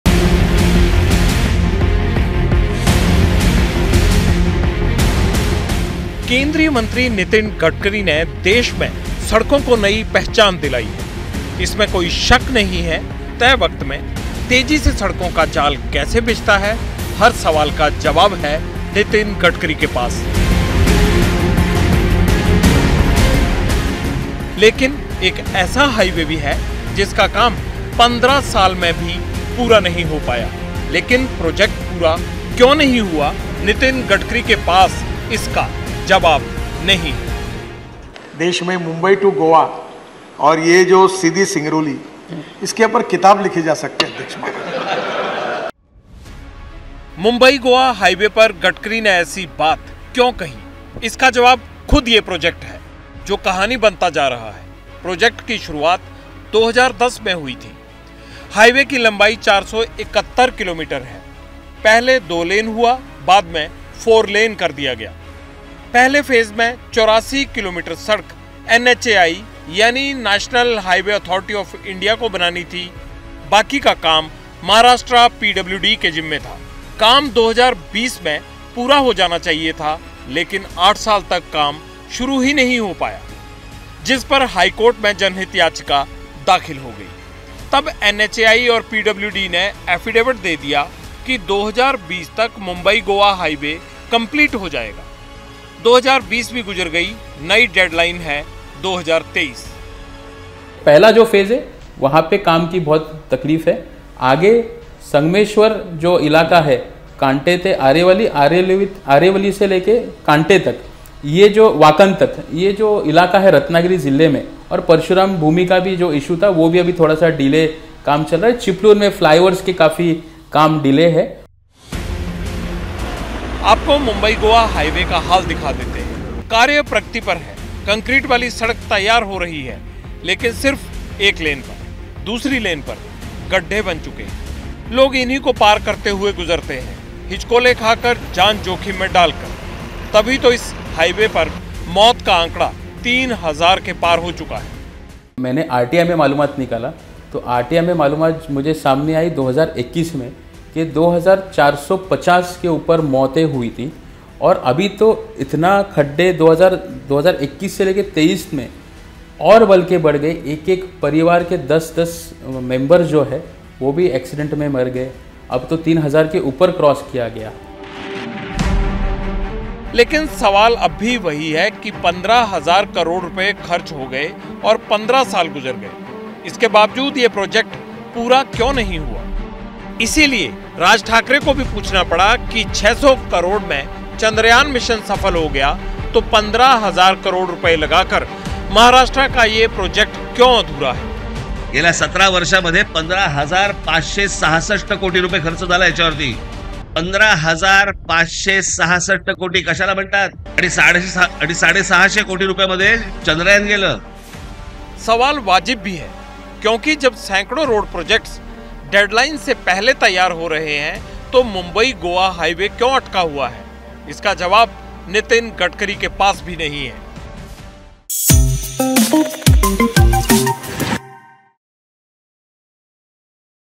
Mumbai Goa Expressway: देश के इस हाईवे ने Nitin Gadkari के ऊपर उठा दिए हैं सवाल | ABP LIVE
नितिन गडकरी देश के वो मंत्री हैं... जिनके काम की तारीफ प्रधानमंत्री मोदी भी करते हैं... सड़कों के मामले में गडकरी ने खूब काम किया है... लेकिन एक रोड प्रोजेक्ट ऐसा भी है...जिसने गडकरी के भी हाथ खड़े करा दिए...डेढ़ दशक बीत गया.. हजारों करोड़ खर्च हो गए.. लेकिन महाराष्ट्र का ये हाईवे पूरा ही नहीं हो रहा...